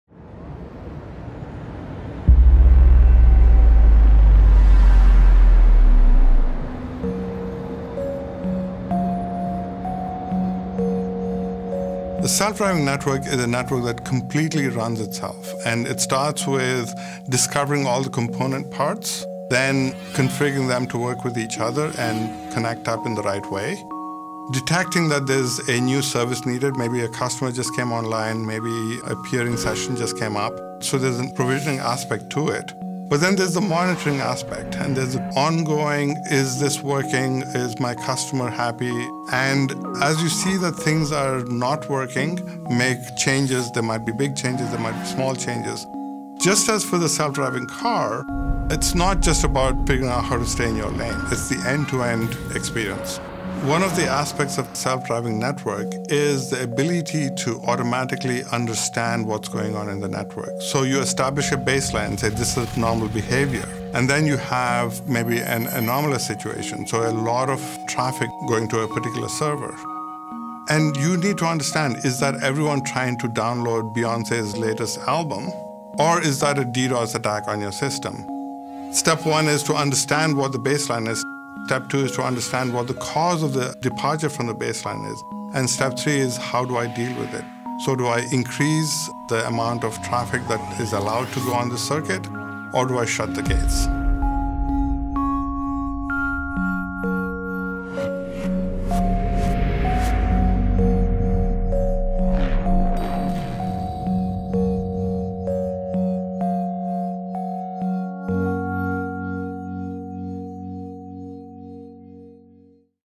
The Self-Driving Network™ Episode 2: What is it?
Juniper product development CTO Kireeti Kompella describes the Self-Driving Network™.